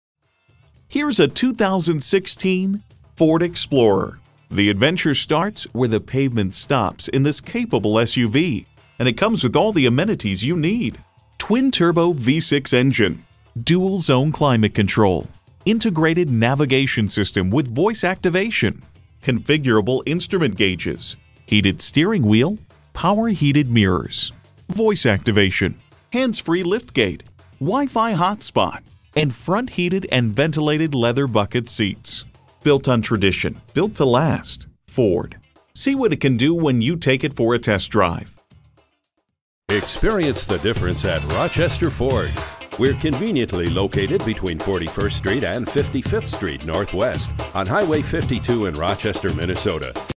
Used 2016 Ford Explorer Rochester MN Winona, MN FA189520 - SOLD
Year: 2016
Make: Ford
Model: Explorer
Trim: Sport
Engine: EcoBoost 3.5L V6 GTDi DOHC 24V Twin Turbocharged
Transmission: 6-Speed Automatic with Select-Shift
Color: Red
Interior: Sport
Mileage: 54914
Stock #: FA189520
VIN: 1FM5K8GT6GGA56522
Recent Arrival! Explorer Sport Red Clean CARFAX. AWD, Power Folding 3RD Row Seat, Heated Steering Wheel, Adaptive Cruise Control & Fwd Collision Warning, Back Up Camera, Heated Seats, Cooled Seats, Leather Seats, Dual Panel Sunroof/Moonroof, Tow Package, Remote Start, Power Liftgate, My Ford Touch, Touch Screen Navigation, Non Smoker, Ambient Lighting, Auto-Dimming Driver's Sideview Mirror, BLIS Blind Spot Information System, BLIS Plus Inflatable Rear Safety Belts Package, Dual 2nd Row Smart Charging USB, Equipment Group 401A, Front 180 Degree Camera w/Washer, Hands-Free Liftgate, Heated Steering Wheel, Inflatable Rear Seatbelts, Memory Feature, Perf Leather-Trimmed Heated/Cooled Bucket Seats, Power Adjustable Pedals, Power Converter 110V Outlet, Power Folding Sideview Mirrors w/Autofold, Power Tilt/Telescoping Steering Wheel, Universal Garage Door Opener, Voice-Activated Navigation System.brbrbrAwards:br * 2016 KBB.com Best Buy Awards Finalist * 2016 KBB.com 5-Year Cost to Own Awards * 2016 KBB.com Brand Image AwardsbrbrStop by, call, or email us today at Rochester Ford. We look forward to earning your business.

Address:
4900 Highway 52 North